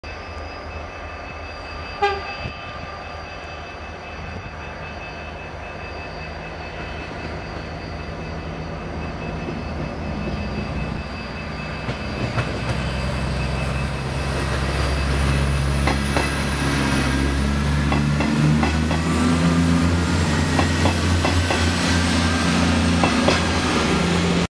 Tren MD Salamanca - Palencia con automotor 599
Salida de Salamanca a las 13:45, llegada a Palencia a las 15:41, todos los dias.
Mi artículo renovado sobre los trenes de la serie 599 de Renfe:
Gracias por vuestras visitas.